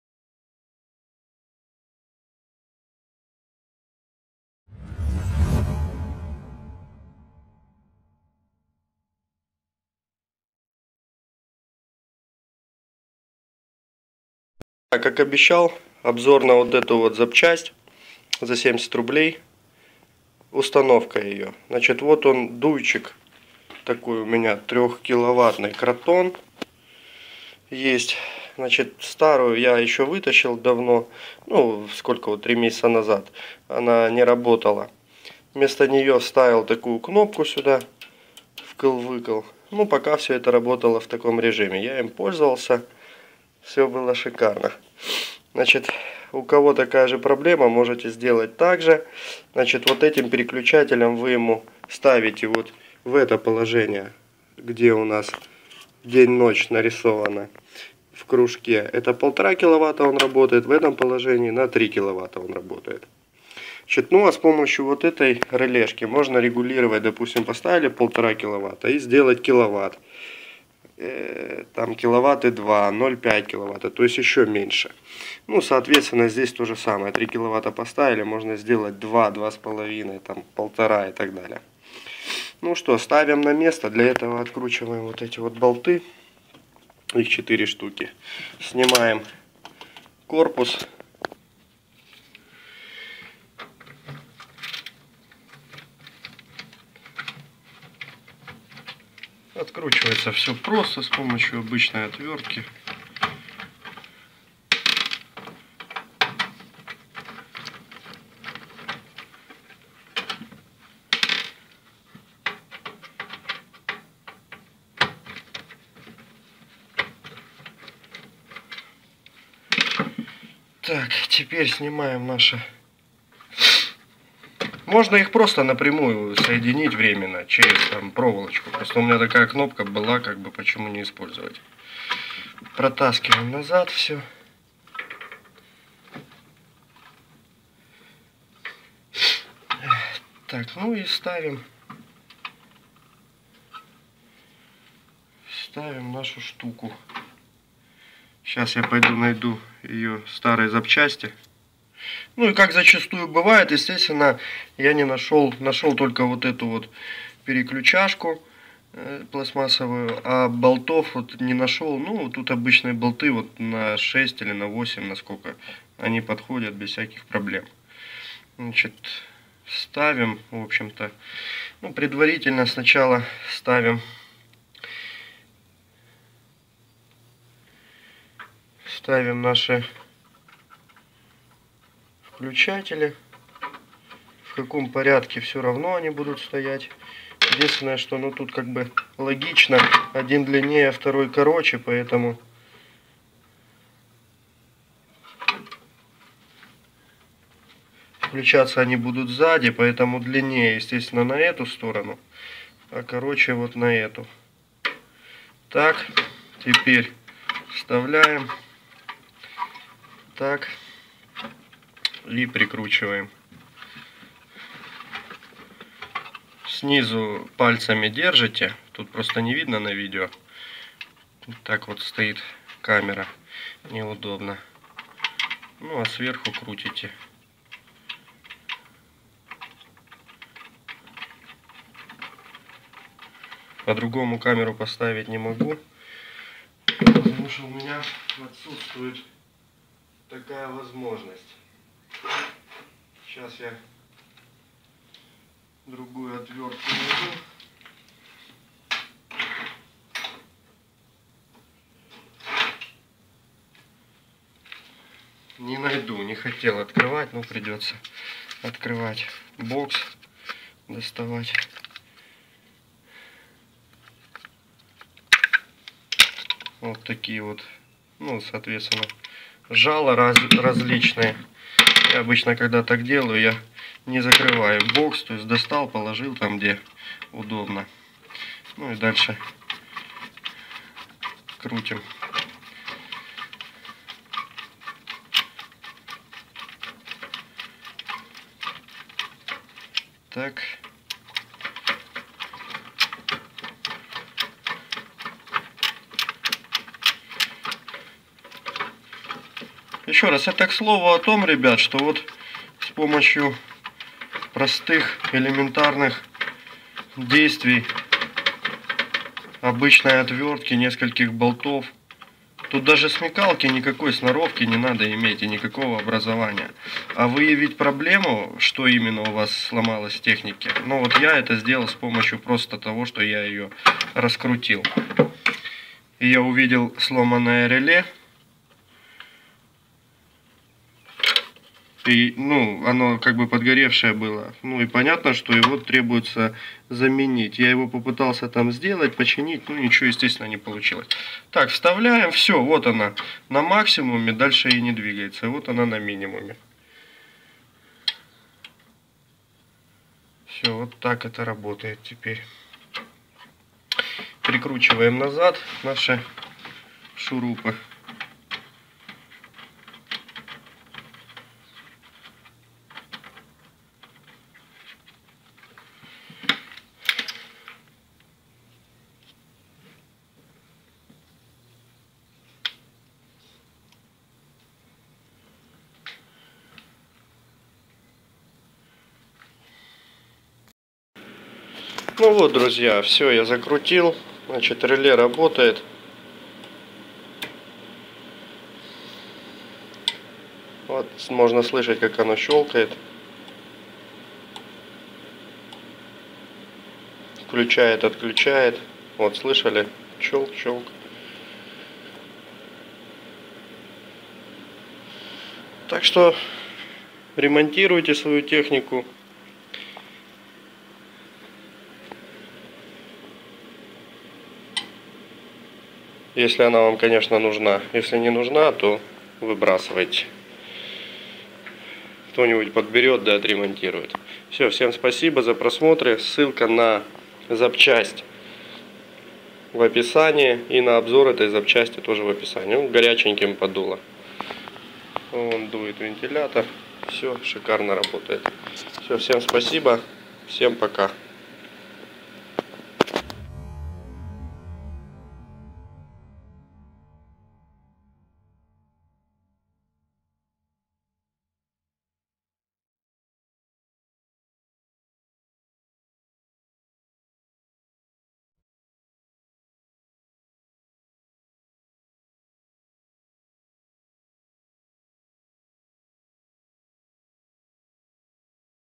Замера реле тепловой пушки "Кратон"
Ремонт (замена реле) тепловой пушки "Кратон"
Автор старается!
Поддержи автора подпиской на канал!